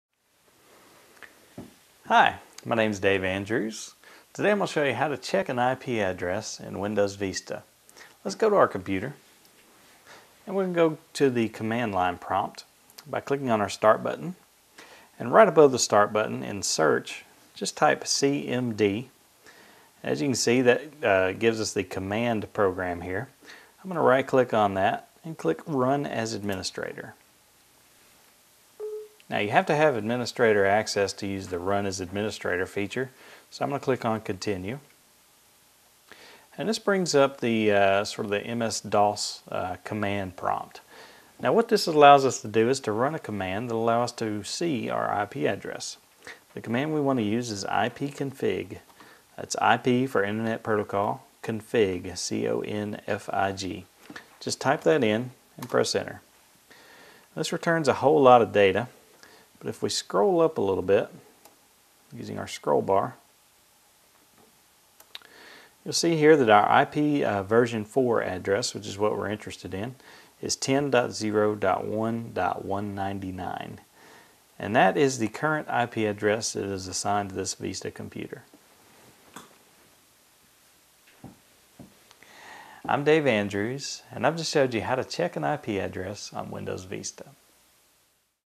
Computer Help & Solutions : How to Check an IP Address in Windows Vista
When using Windows Vista, go to the command line prompt, and run it as an administrator. Type "ipconfig" into the MS DOS command prompt to see the IP address with help from a software developer in this free video on IP addresses.